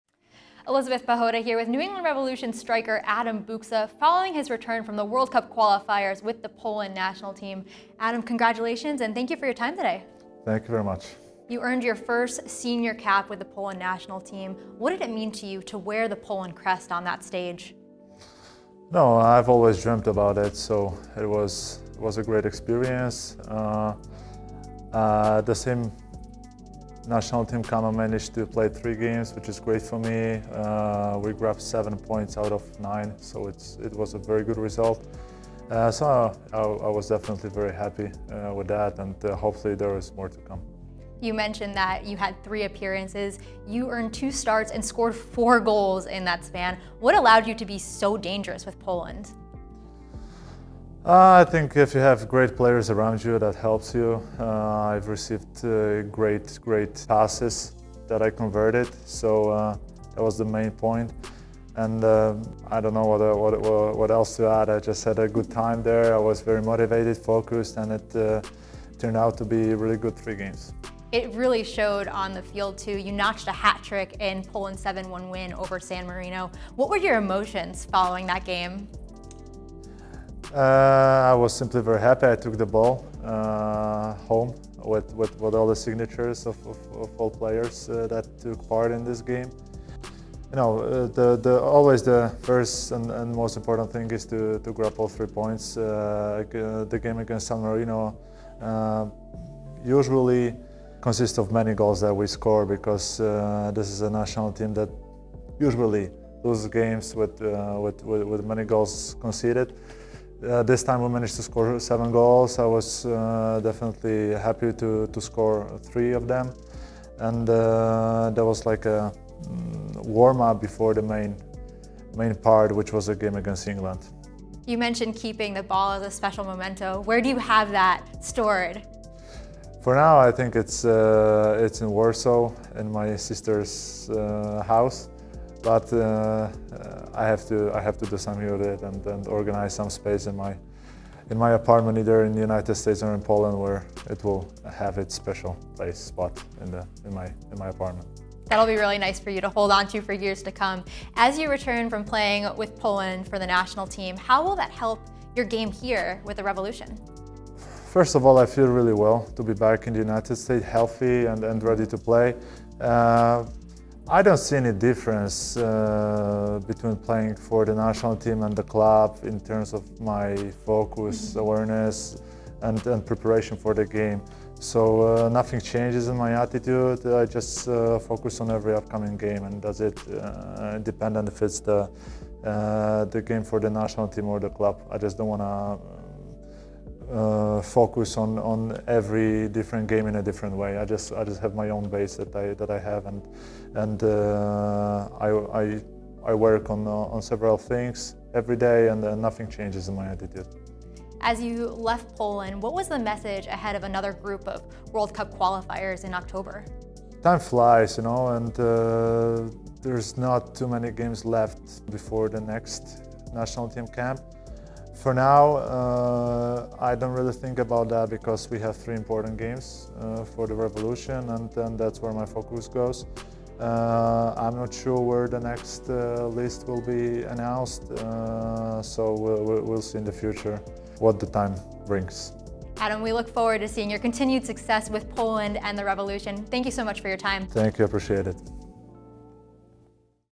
Adam Buksa Discusses Current and Future Polish National Team Goals
Adam Buksa sat down with Elizabeth Pehota to talk about his most recent experience with the Polish National Team during European World Cup Qualifiers, notching his first goal and what lies ahead for him ahead of the 2022 World Cup.